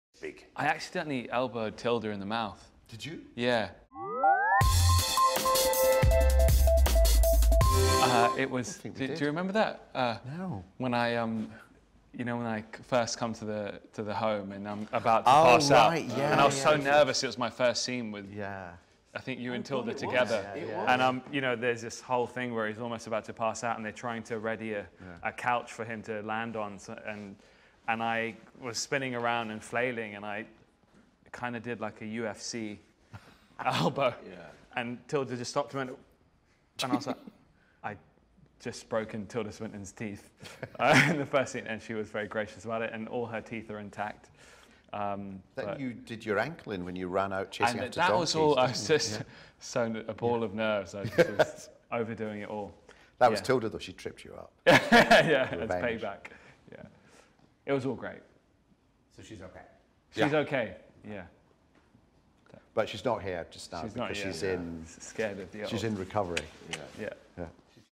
Dev Patel Accidentally 'UFC Elbowed' Tilda Swinton On Set | PeopleTV | Entertainment Weekly
Dev Patel talks to us about his first day of filming 'The Personal History of David Copperfield' with Tilda Swinton during the 2019 Toronto International Film Festival.